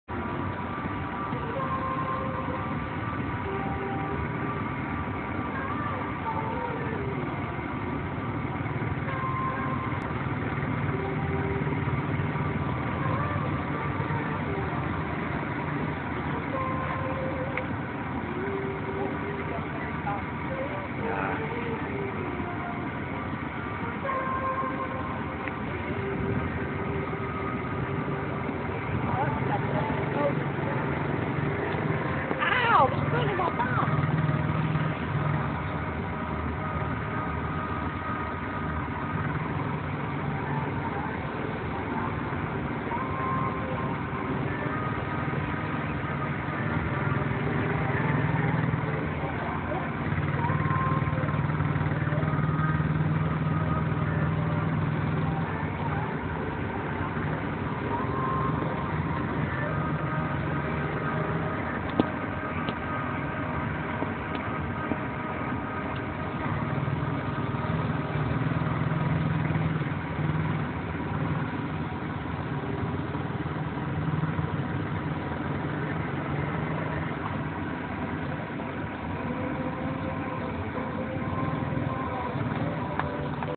Quads in spain
Just for fun mini quads in fuerteventura